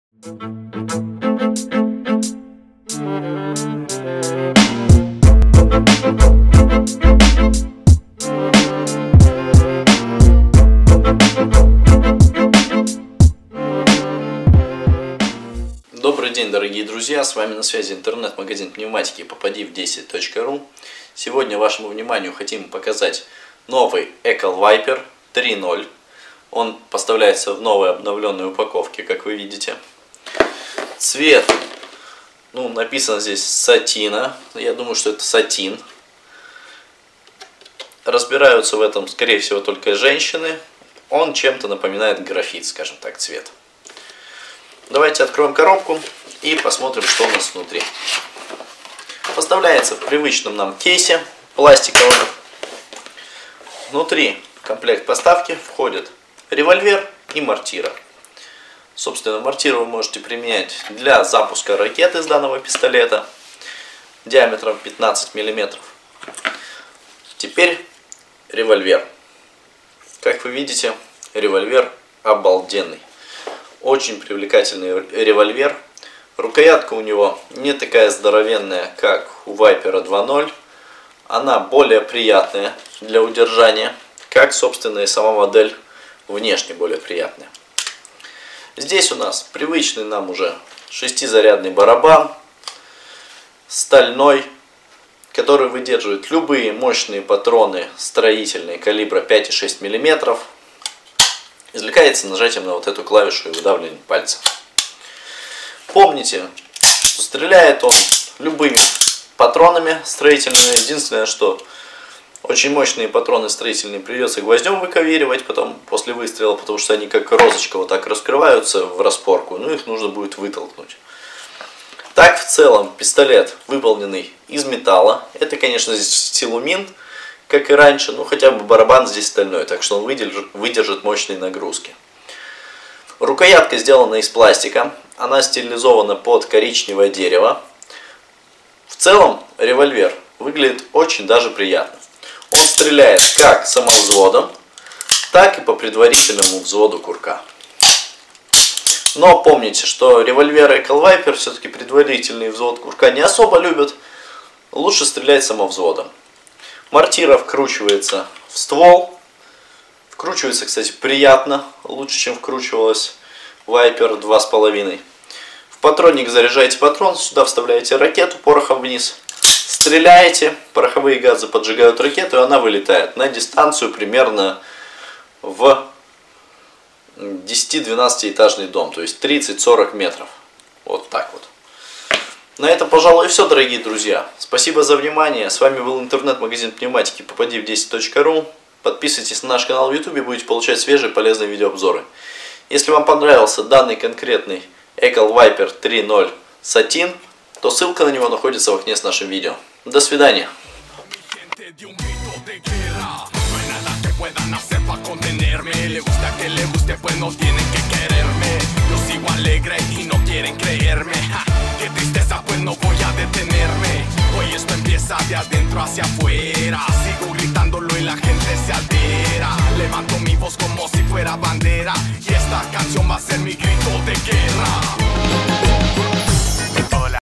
Сигнальный револьвер Ekol Viper 3.0 "Satine". Купить popadiv10.ru
В данном видео мы покажем Вам сигнальный револьвер Ekol Viper 3.0 в цвете (Сатин), расскажем о его технических характеристиках. Если вам понравился данный револьвер ссылки на него в видео и под описанием.